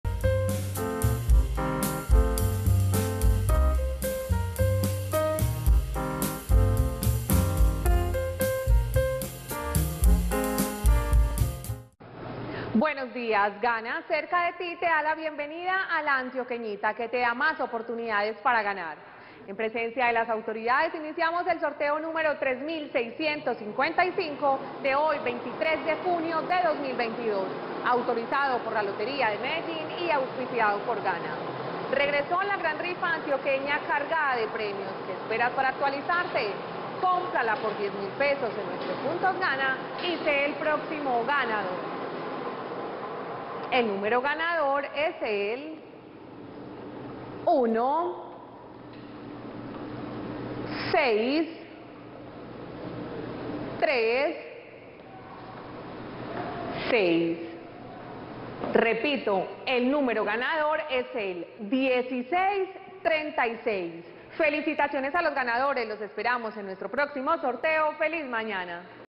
LA ANTIOQUEÑITA 1 Resultado del JUEVES 23 DE JUNIO de 2022 ✅🥇🔥💰 NÚMERO GANADOR
Hola buenos días, te presentamos los resultados de las loterías y chances en Colombia, vamos con el Resultados del Chance Antioqueñita 1 del JUEVES 23 DE JUNIO de 2022.